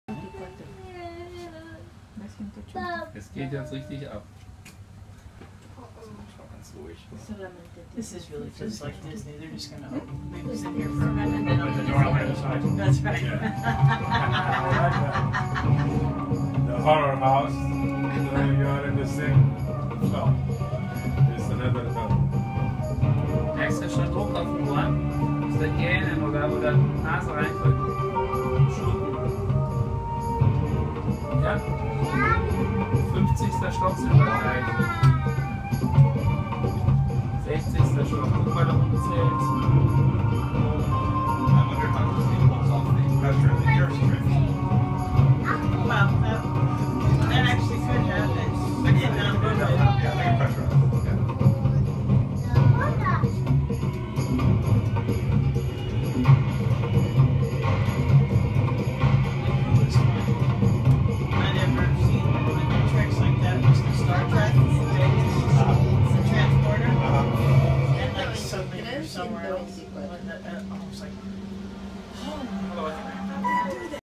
burj khalifa elevator ride
riding the elevator to the observation deck. 124 floors in 60 seconds.